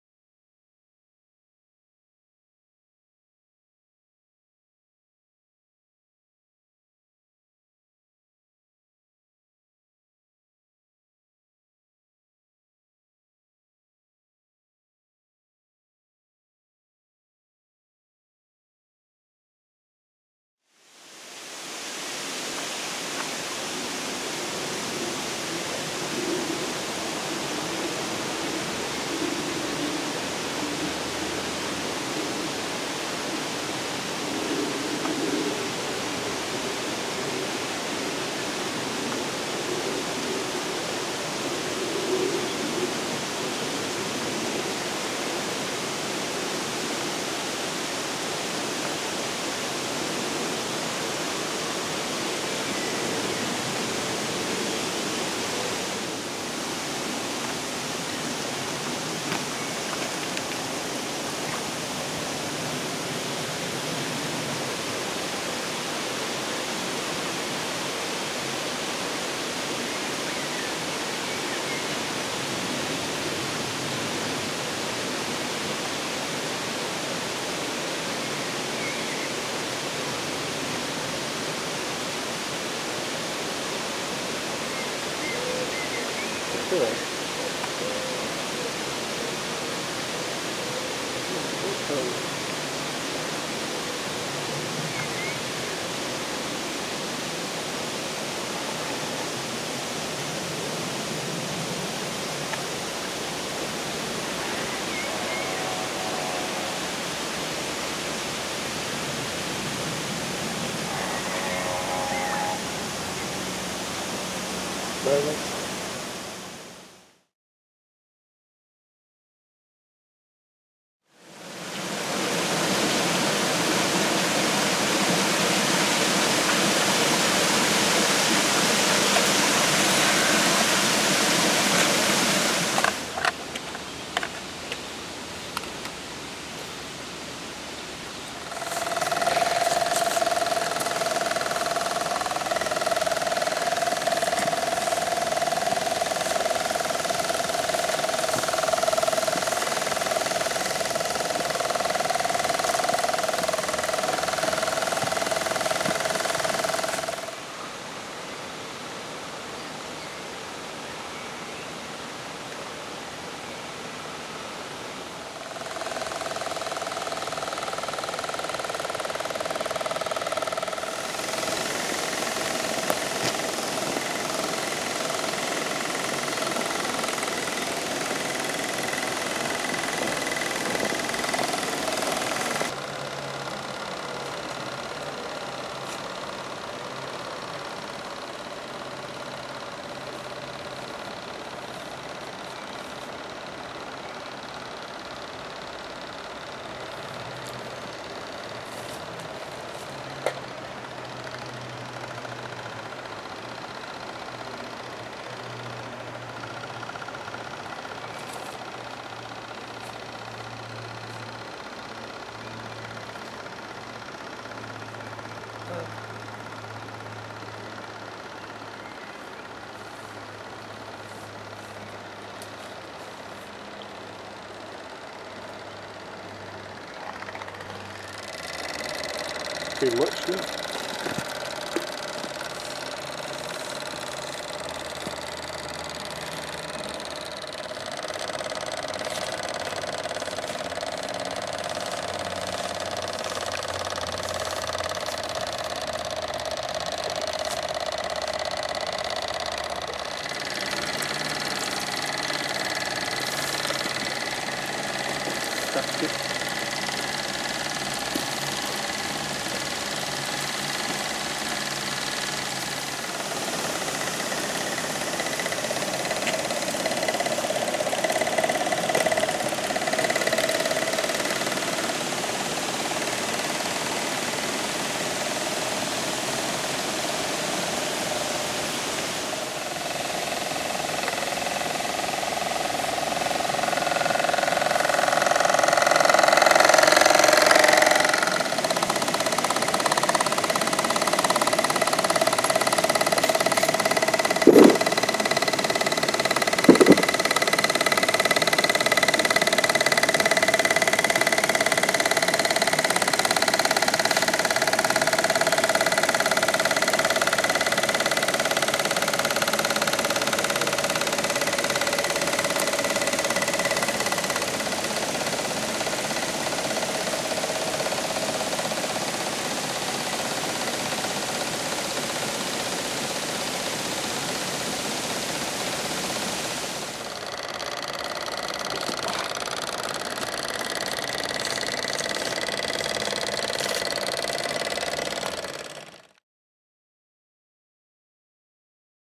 B.O.A.T.S: BLACK COUNTRY OUTSIDE AQUA TOY SOCIETY, INAUGURAL MEETING, MAY 2016
The first meeting of the Black Country Outside Aqua Toy Society, May 2016, featuring a commercially available RNLI Severn Class Lifeboat RTR model and a rather nice Chart Steam Explorer 2. Copyright Playing Around! (2016).